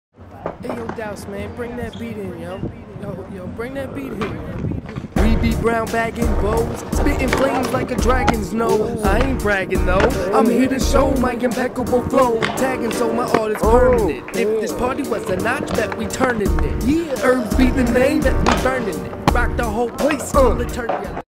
Laurel Clips of the day!
Laurel JAM Session - Laurel Skate Park - Herbcrew Getting our skate on! Rockin out with local skaters